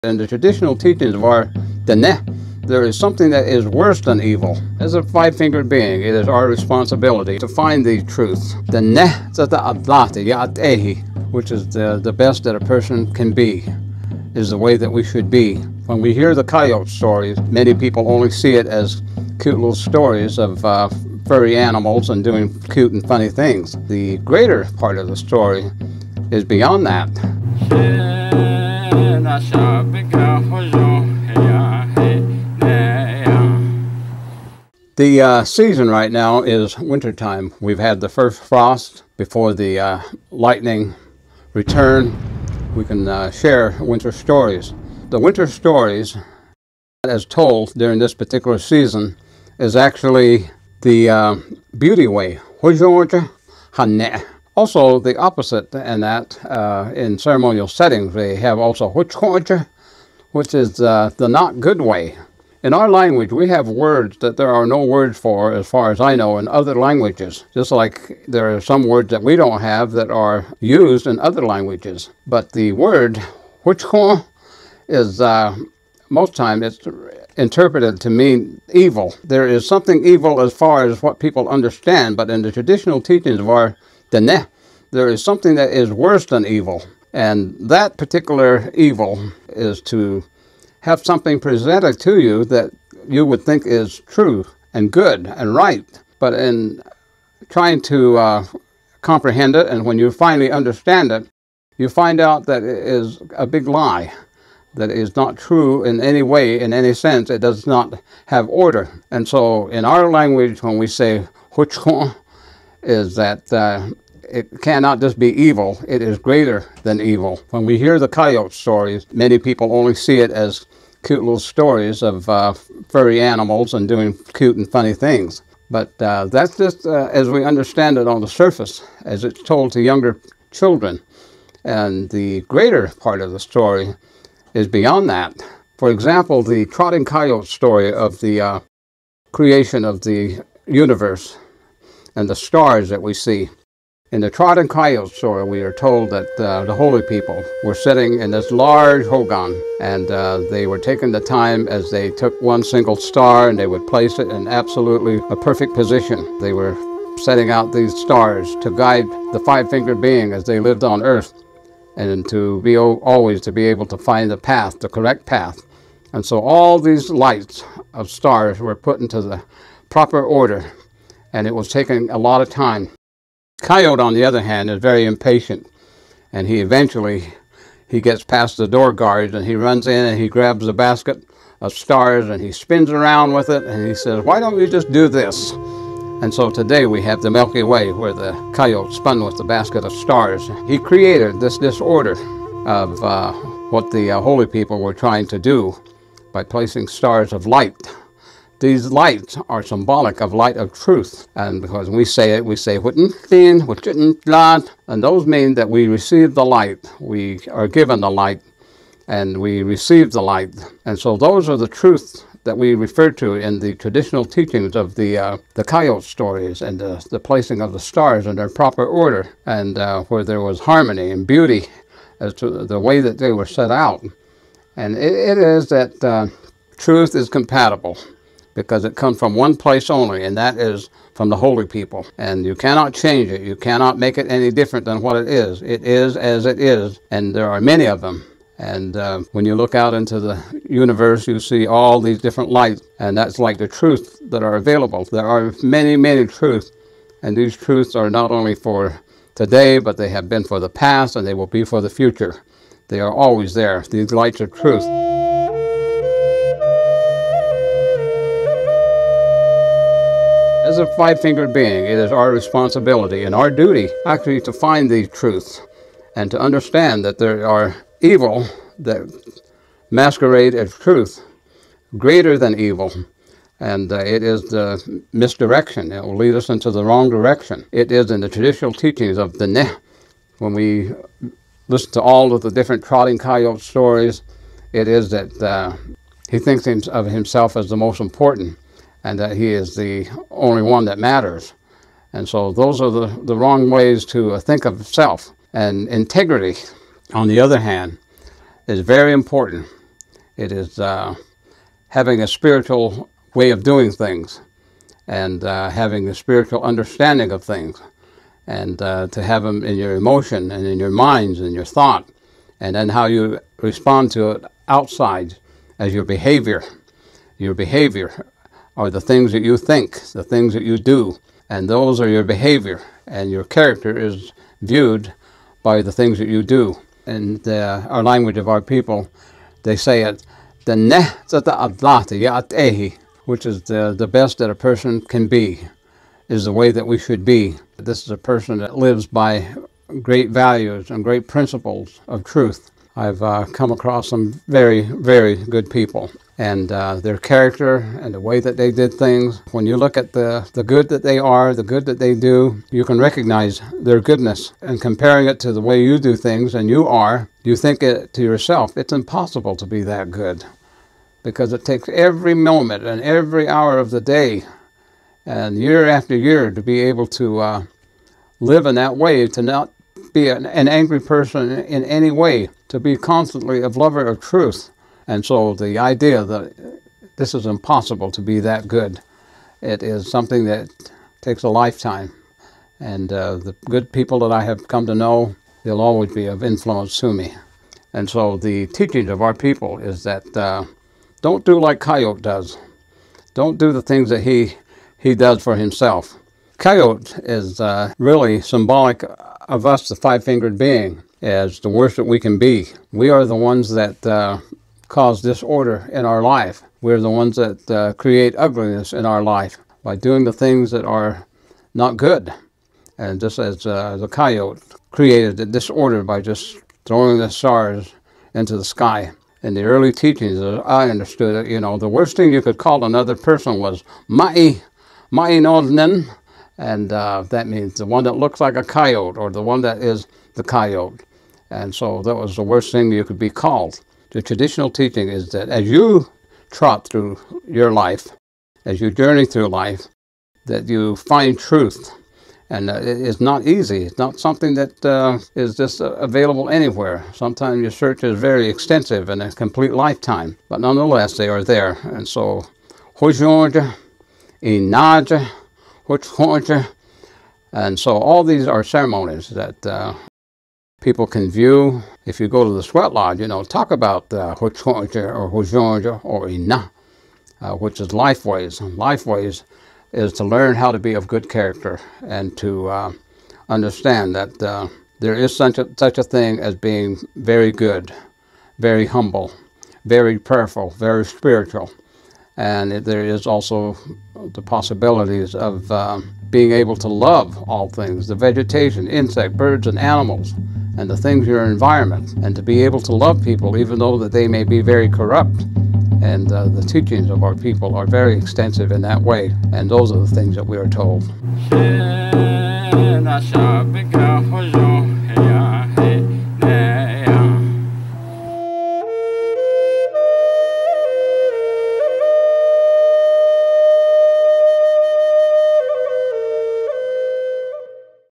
How The Stars Were Placed, What it Means for You and What is the Way of Integrity?
In this week’s video… Navajo historian Wally Brown teaches about Coyote and how he frustrated the work of the Holy People when they were creating the universe and placing the stars in the sky.

People think coyote stories are about cute furry creatures that do funny things. But the true meaning is beyond that.

Traditionally the way of integrity is something everyone should strive for.

Heed the lesson and don’t be like Coyote… Instead be thoughtful, prayerful… Walk in Beauty.